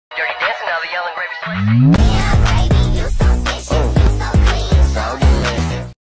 When Pluto meets the dwarf planets for the first time // planethumans // Gacha life
Characters (planets) :

Planethumans Pluto
Planethumans Ceres
Planethumans Makemake
Planethumans Haumea
Planethumans Eris
(Ignore) :

Planets
Solar System
Meme
Gacha
Dwarf planets